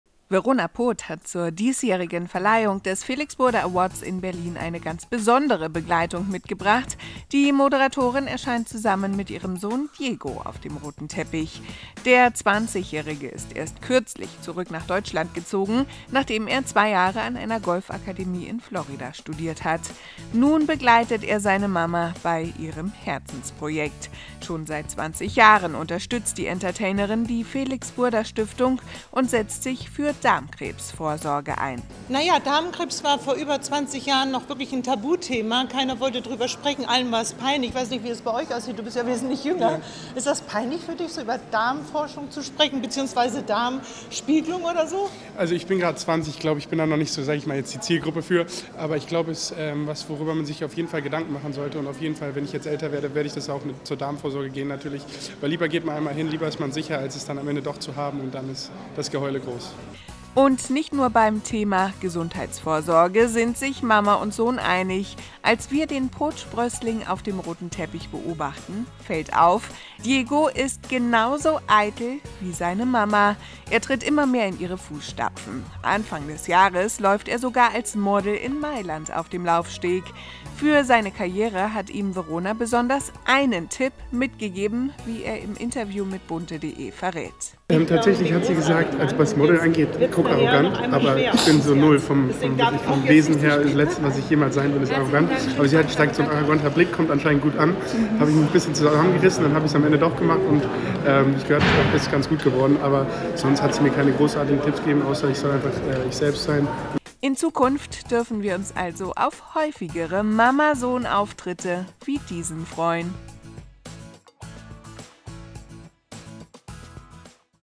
Verona Pooth - Erwischt: Sohn Diego kommt ganz nach seiner Mama
Verona Pooth hat zur Verleihung des Felix Burda Awards in Berlin eine ganz besondere Begleitung mitgebracht: ihren Sohn Diego. Der 20-Jährige kommt ganz nach seiner Mama.

Impressum:
Burda Forward
St.-Martin-Straße 66
81541 München